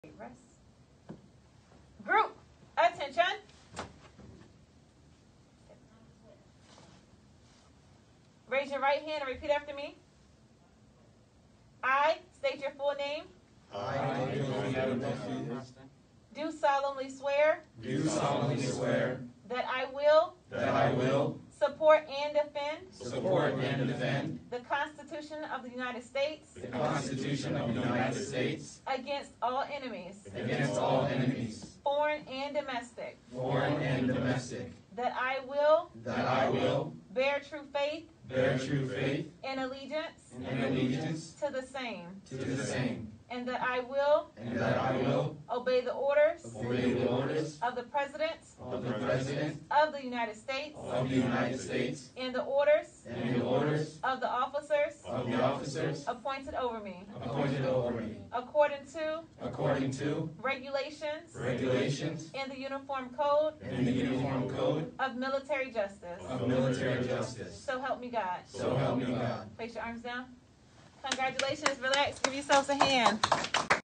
I SWORE INTO THE U.S. ARMY! (Oath of Enlistment) Nov. 25th 2020
I appreciate all the love & support on my channel!